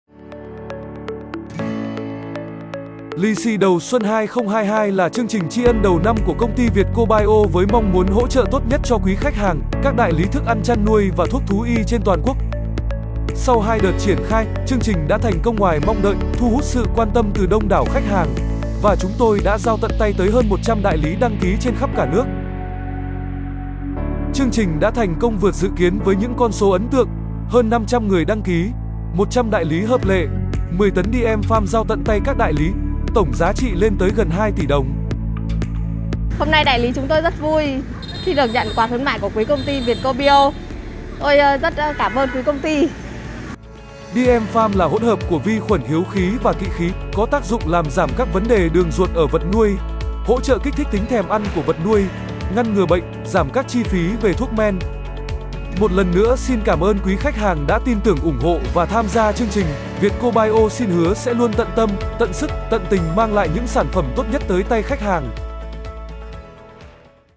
Tổng kết chương trình Promotion Mừng xuân 2022 - Tổng giá trị quà tặng lên đến 2 tỷ đồng
“Lì xì đầu xuân 2022” là Chương trình tri ân Vietko Bio triển khai với mong muốn hỗ trợ tốt nhất cho quý khách chung tay vượt dịch Covid 19 dành cho các đại lý thức ăn chăn nuôi và thuốc thú y trên toàn quốc. Triển khai từ ngày 14/02/2022- 13/03/2022, Chương trình đã thu hút sự quan tâm từ đông đảo khách hàng.

Những cái bắt tay chúc mừng, những lời thăm hỏi động viên, những nụ cười của khách hàng là nguồn động lực to lớn giúp chúng tôi hoàn thành chương trình.

🏤 Địa chỉ: Số 11 đường Nguyễn Đăng, khu 2, Phường Suối Hoa , Thành phố Bắc Ninh, Tỉnh Bắc Ninh, Việt Nam